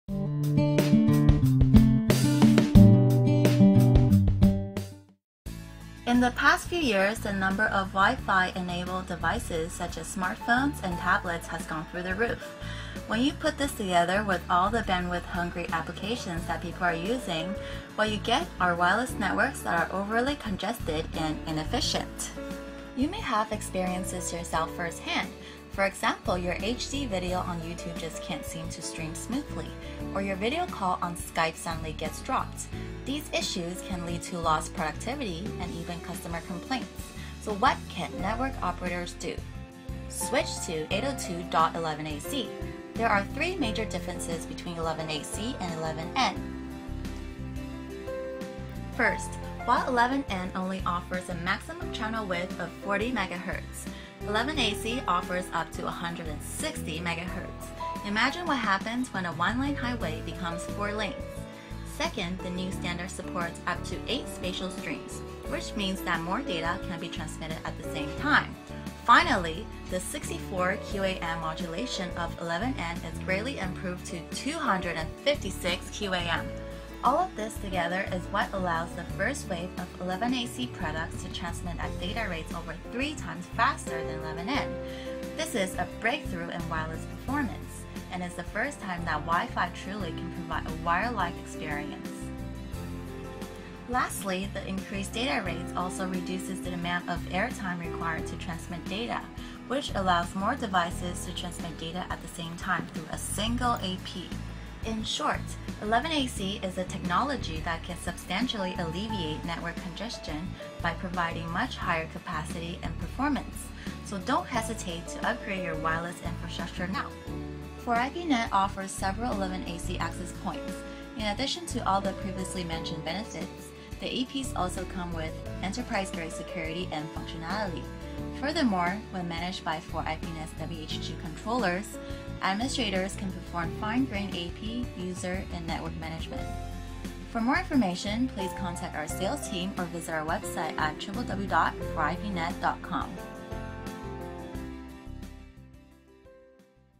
Introduction to 802.11ac: The Future of Wi-Fi
4ipnet introduces the latest IEEE Wi-Fi standard 802.11ac. This video illustrates the differences between 802.11ac and 802.11n. In addition, 4ipnet has launched AC1750 enterprise-grade access points that are feature-rich and cost-effective.